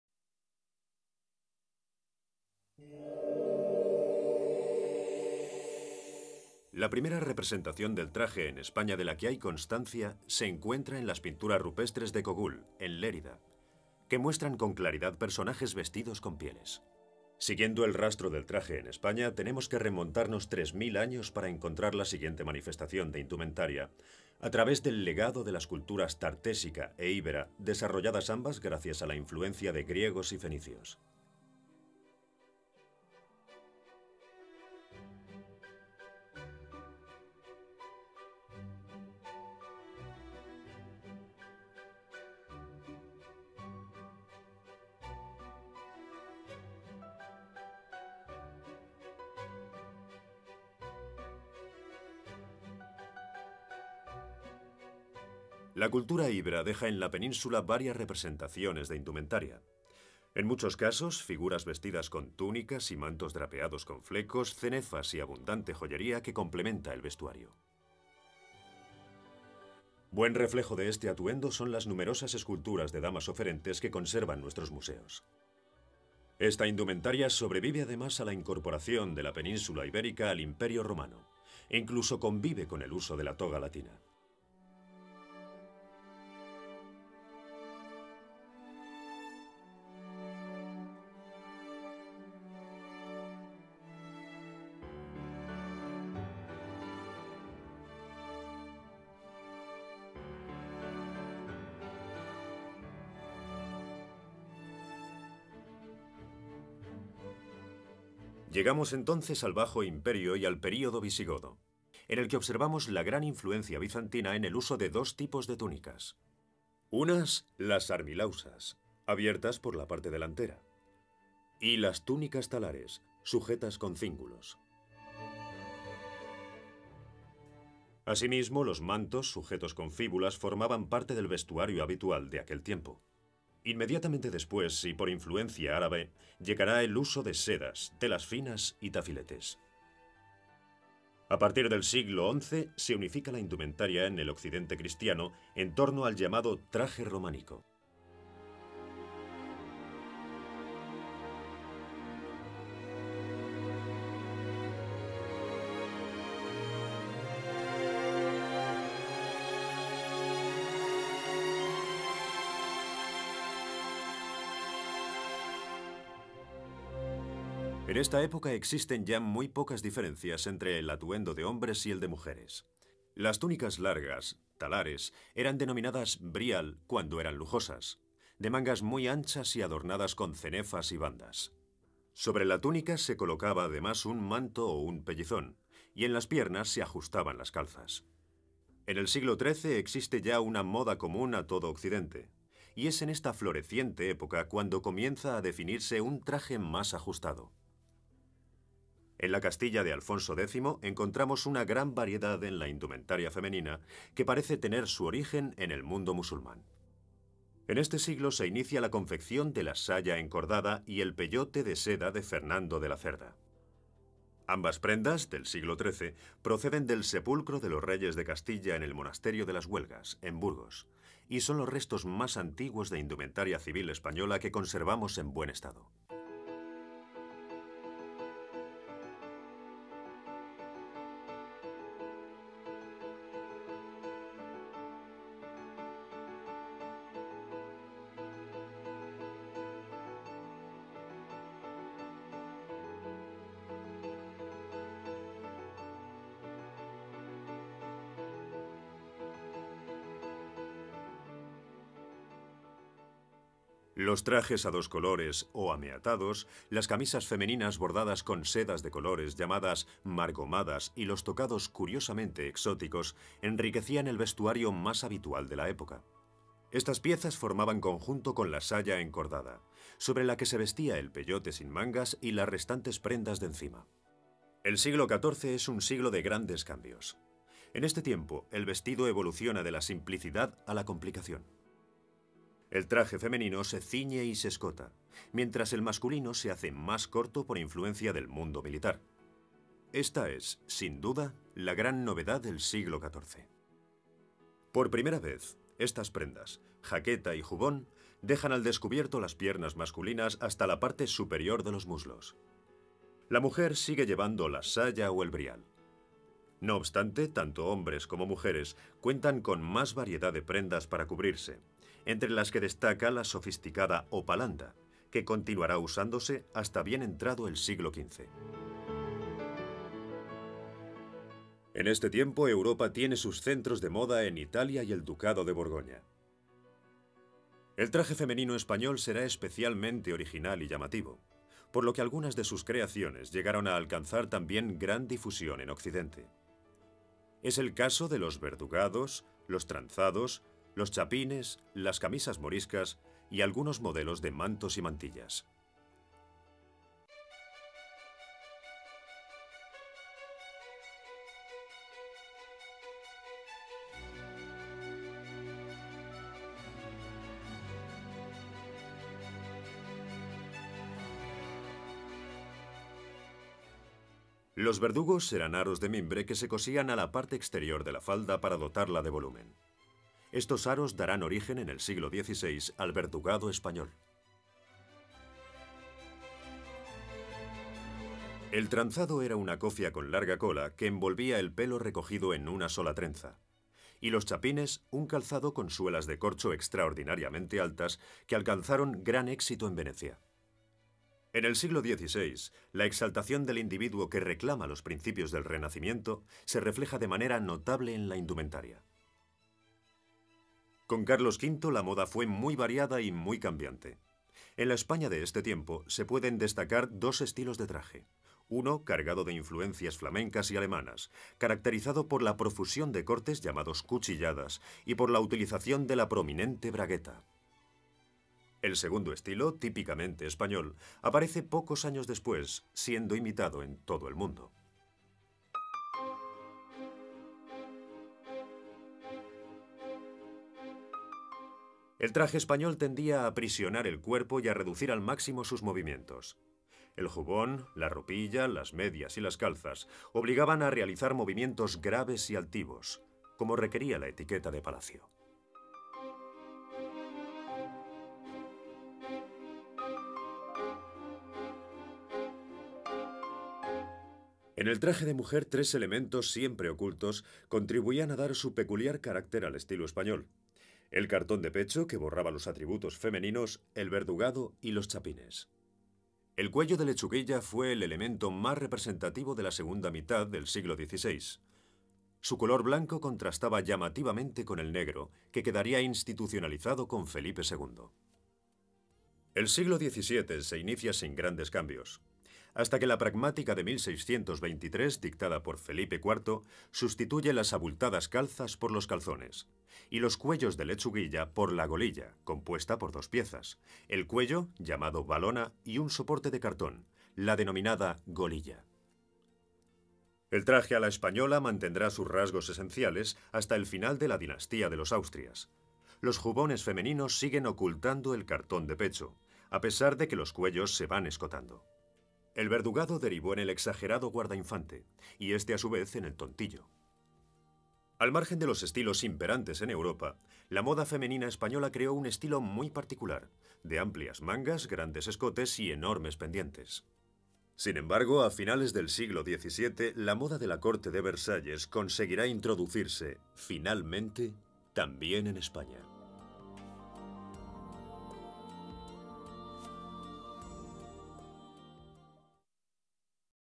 Indumentaria histórica en España. Desde los orígenes hasta el s. XVII
Son muy pocas las prendas que conservamos de épocas anteriores al siglo XVII. Por lo tanto para el estudio de la indumentaria debemos recurrir a otras fuentes como el arte. La escultura aporta la representación en 3 dimensiones del vestido, y la pintura contribuye al conocimiento del color. Este audiovisual está disponible en el Museo del Traje de Madrid, en la sala llamada ?Tiempos Lejanos?, donde conocemos la evolución de la indumentaria desde los testimonios de pinturas rupestres, la cultura íbera, pasando por el Imperio romano y la Edad Media, hasta finalizar en el siglo XVII.